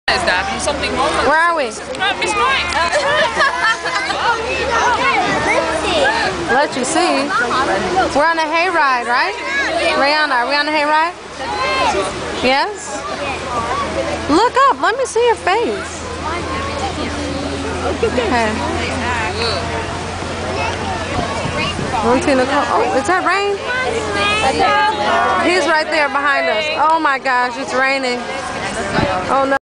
Noah and Nana
Sept 2009 on a hay ride